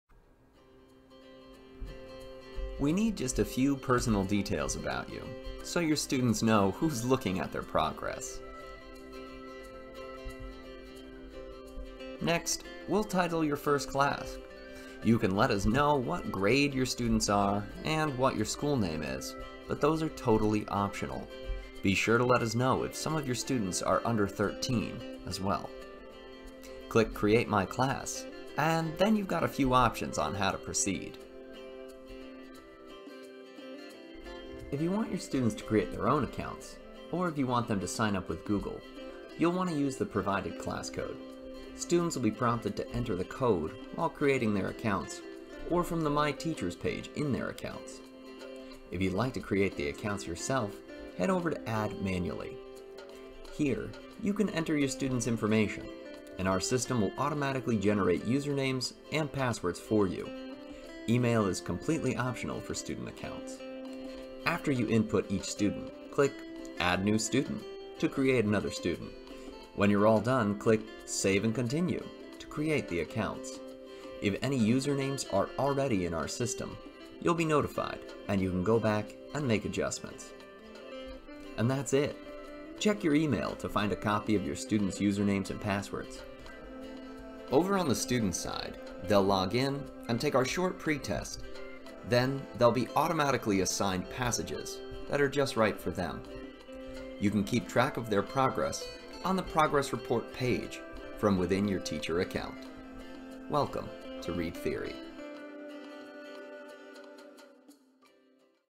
Teacher Guide - Set Up Your ReadTheory Teacher Account
A quick guide on how to set up your teacher account. Learn how to create a class and how to add students to the class.